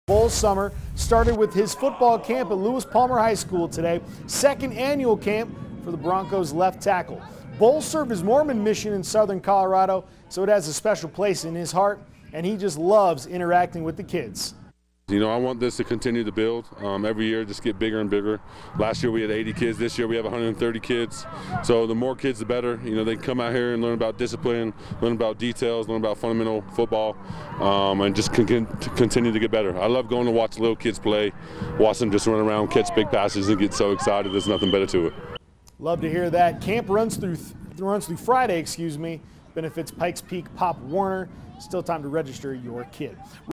Garett Bolles Camp begins second year in Southern Colorado
The Broncos left tackle brought his football camp back to Southern Colorado for the second straight year, benefiting Pikes Peak Pop Warner.